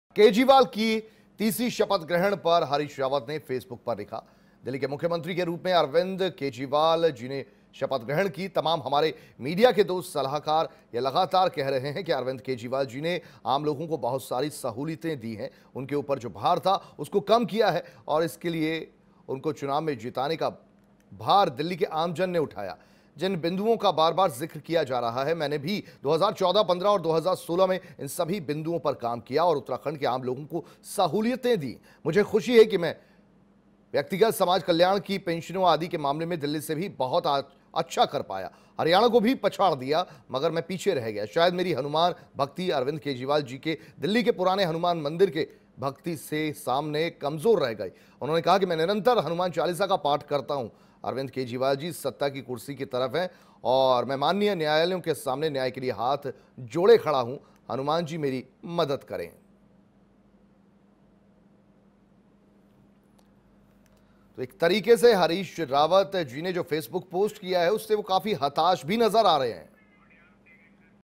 BreakingNews- केजरीवाल के शपथ ग्रहण पर हरीश रावत की पोस्ट R9TV
केजरीवाल के तीसरी शपथ ग्रहण पर हरीश रावत ने फेसबुक पर लिखा कि  ..  दिल्ली के मुख्यमंत्री के रूप में अरविंद_केजरीवाल जी ने शपथ ग्रहण की.. तमाम हमारे मीडिया के दोस्त लगातार कह रहे हैं कि,  अरविंद केजरीवाल जी ने आम लोगों को बहुत सारी सहूलियतें दी..